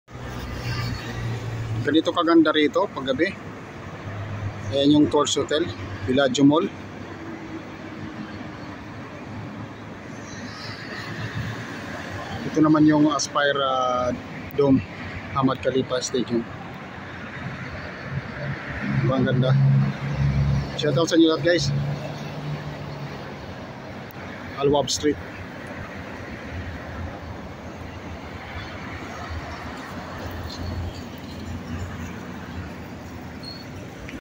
QATAR KHALIFA STADIUM | ASPIRE DOME | TORCH HOTEL
A very unique and artistic TORCH HOTEL, KHALIFA STADIUM, ASPIRE DOME IN QATAR.
 THANKS FOR YOUR Wonderful.